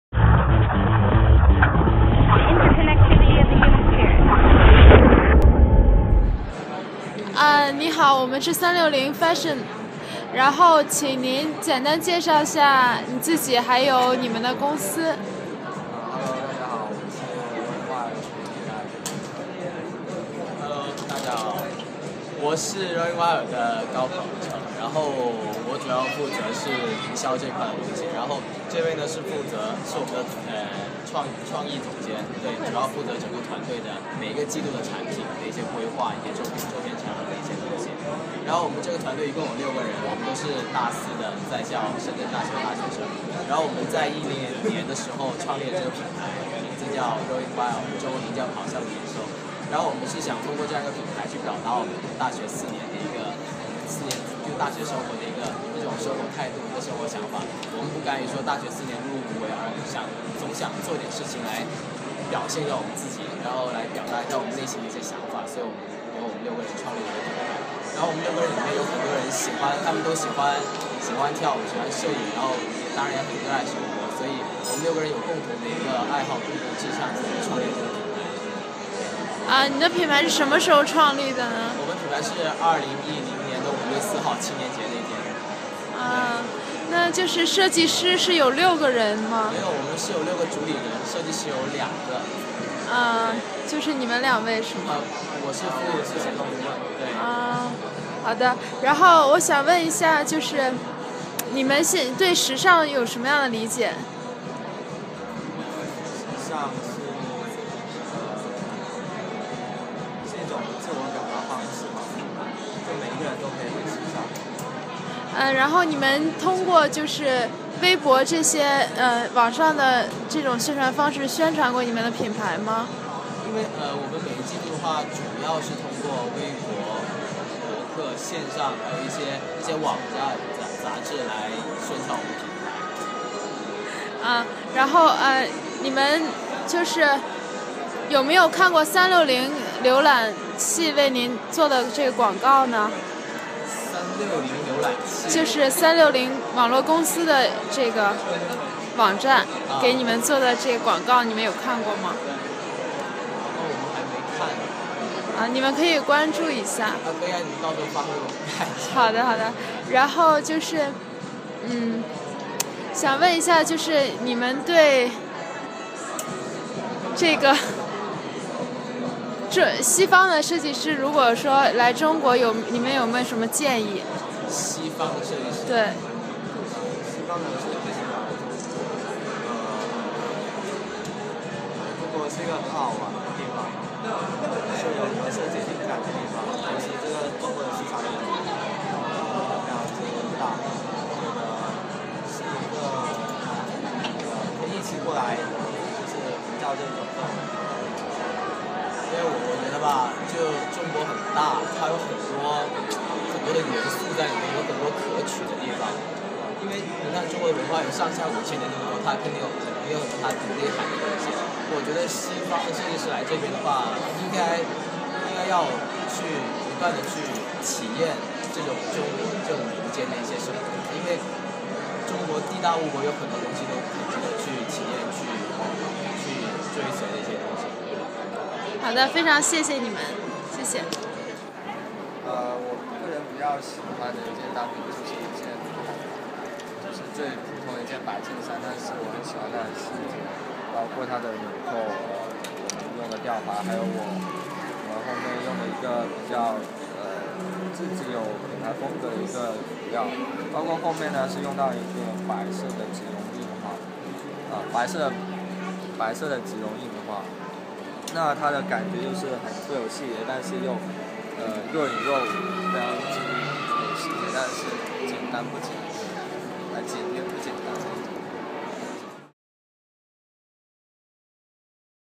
CHIC Young Blood 2012 interview 9 with a great, young and talented designer!
360Fashion Network interview 09 with one of the CHIC Young Blood designers at the CYB 2012 in Beijing,China. We, the 360Fashion Network team really enjoyed this amazing week full of fashion and new brands at the CYB 2012. Next year we will definitely be there and we hope you will be there too!